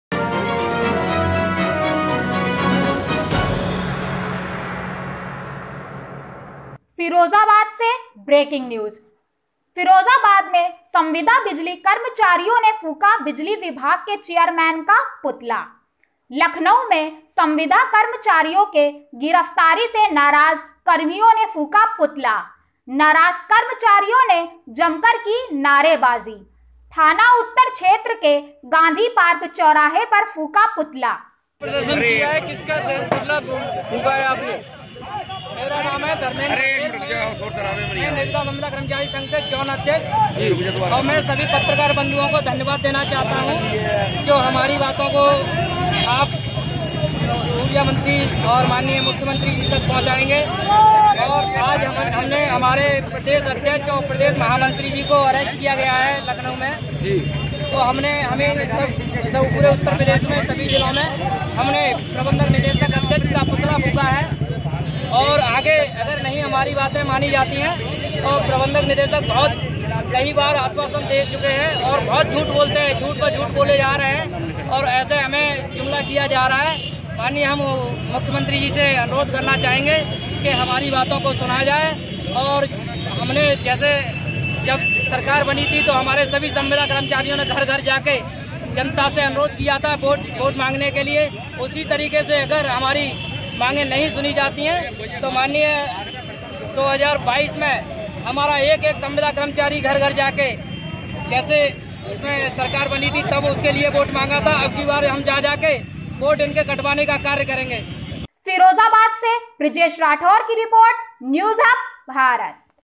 Firozabad: संविदा बिजली कर्मचारियों ने फूंका बिजली विभाग के चेयरमैन का पुतला | News Hub Bharat
About this video-फिरोजाबाद में संविदा बिजली कर्मचारियों ने फूंका बिजली विभाग के चेयरमैन का पुतला

लखनऊ में संविदा कर्मचारीयो के गिरफ्तारी से नाराज कर्मियों ने फूंका पुतला

नाराज कर्मचारियों ने जमकर की नारेबाजी

थाना उत्तर क्षेत्र के गांधी पार्क चौराहे पर फूंका पुतला